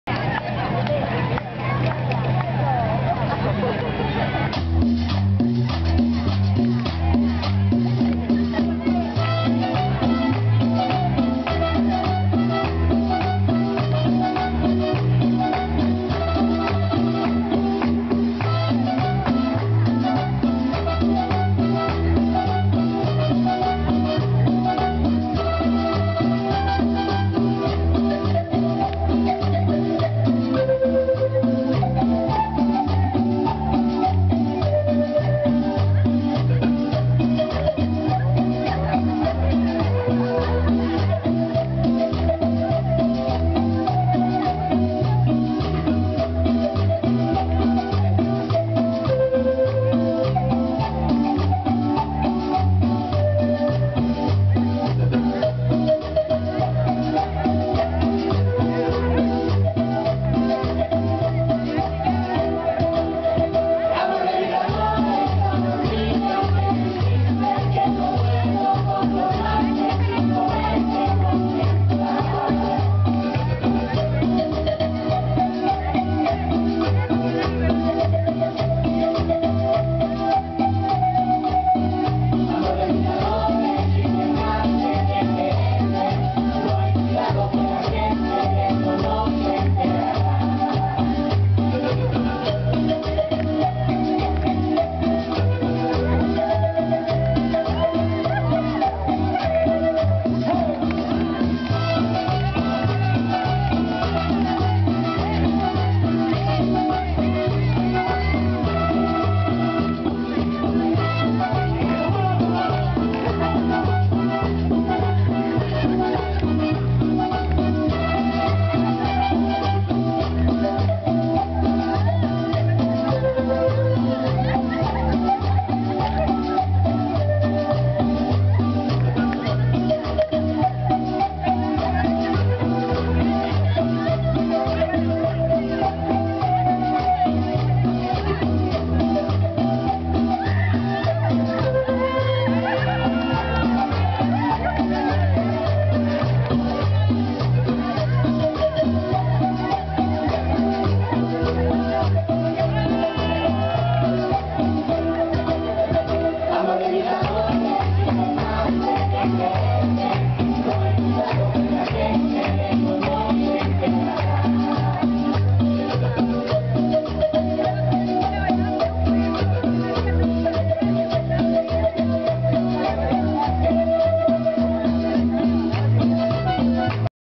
Domingo laguna del nainari 2012 - 3 parte
se la rifo el viejito bailarin  fue el alma de la fiesta cumbiera